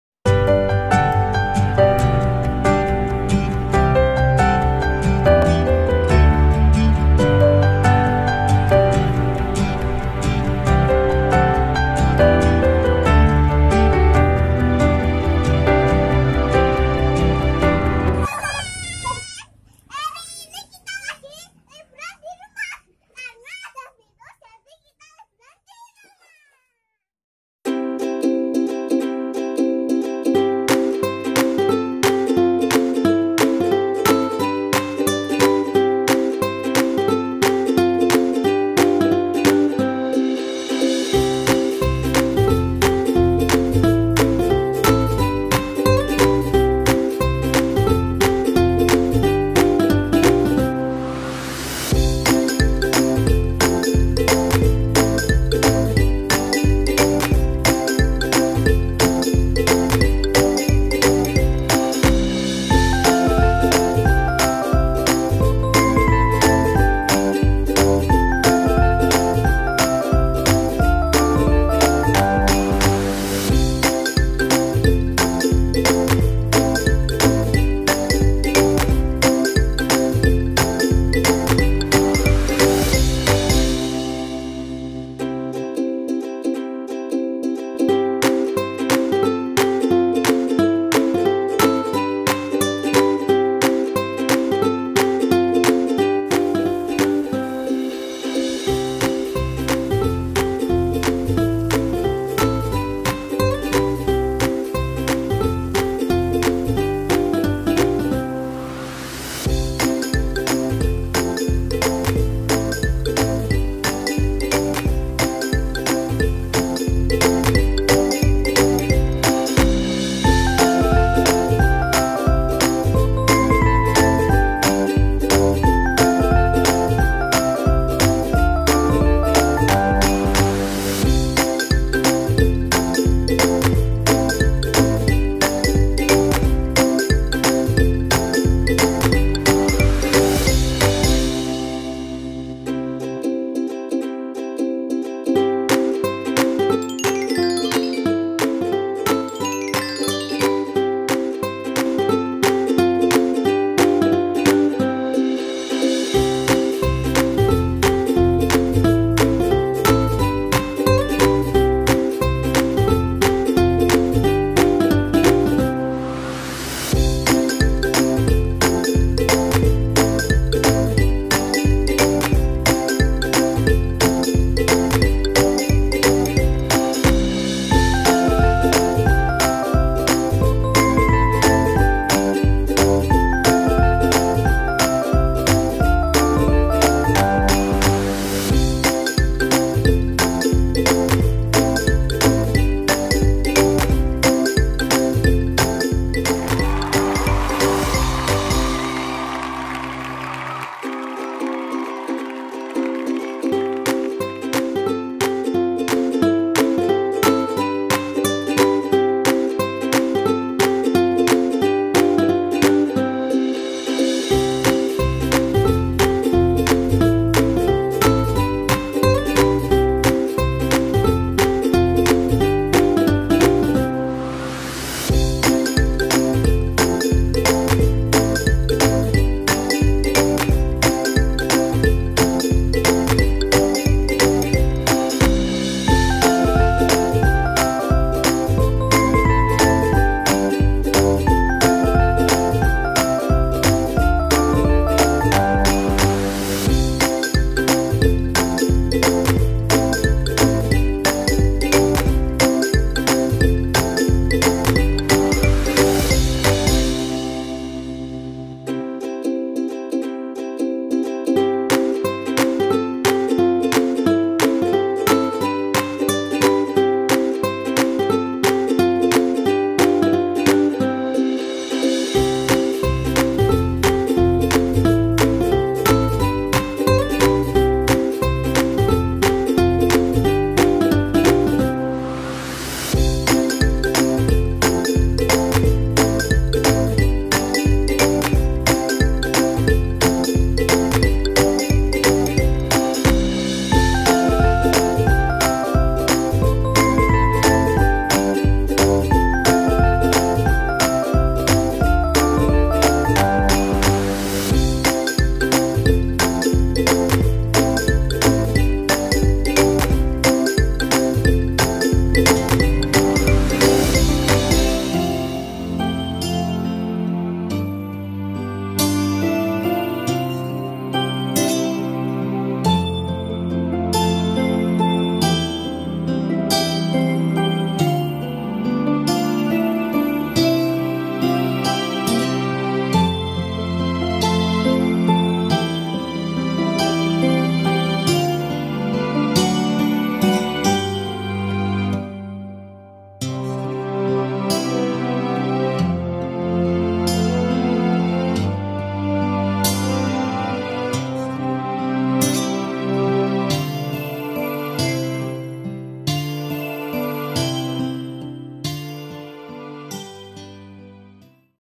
Berenang di 🏠 🏠 🏠
Efek virus Corona Ngajak berenang y berenang di ember saja